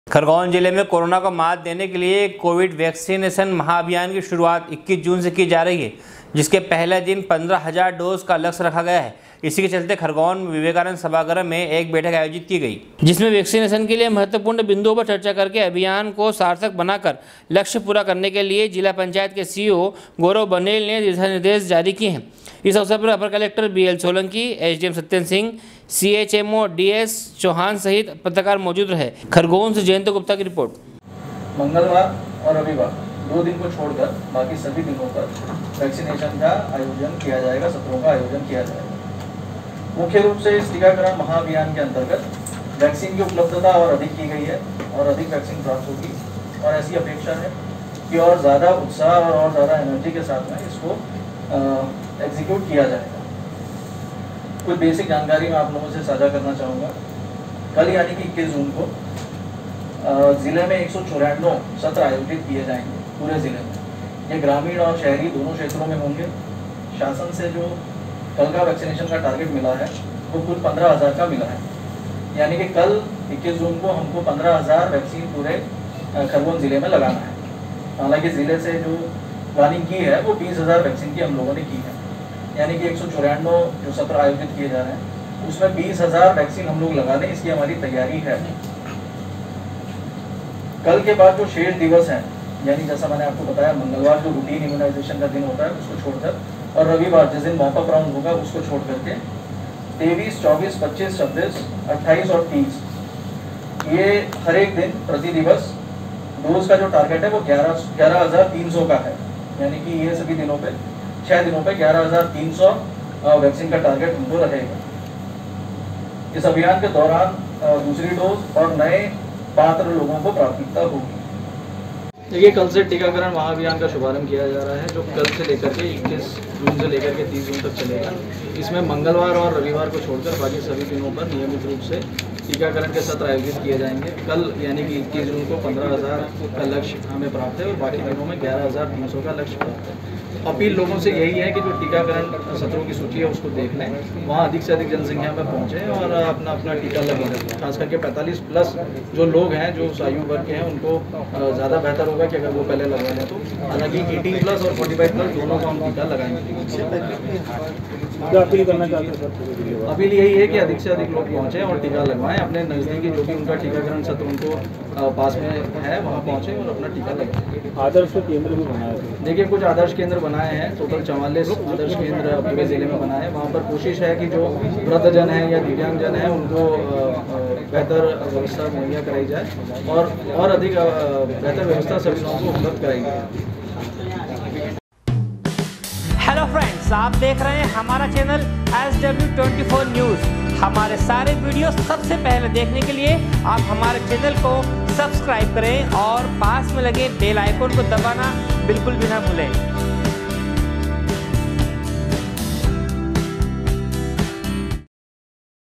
khargone- खरगोन जिले में कोरोना को मात देने के लिये कोविड वेक्सिन महाअभियान की शुरुआत 21 जून से
खरगोन जिले में कोरोना को मात देने के लिये कोविड वेक्सिन महाअभियान की शुरुआत 21 जून से की जा रही है जिसके पहले दिन 15000 डोज का लक्ष्य रखा गया है इसी के चलते खरगोन के विवेकानन्द सभागृह में एक बैठक आयोजित की गयी। खरगोन से जयंत गुप्ता की रिपोर्ट